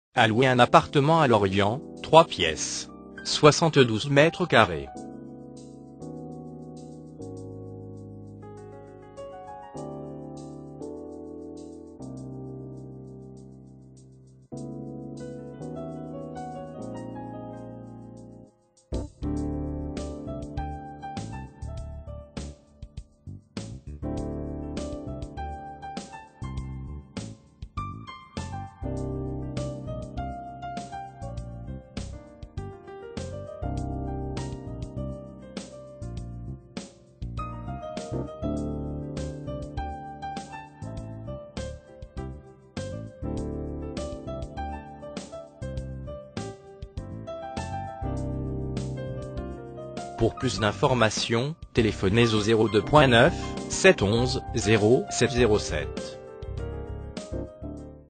Location Appartement LORIENT 56100 - 72 m2
Cliquez sur ce lien pour en savoir plus (prix, contacts)

A louer à LORIENT un appartement de 72 m2.

Annonce immobilière : Location Appartement LORIENT 56100 Morbihan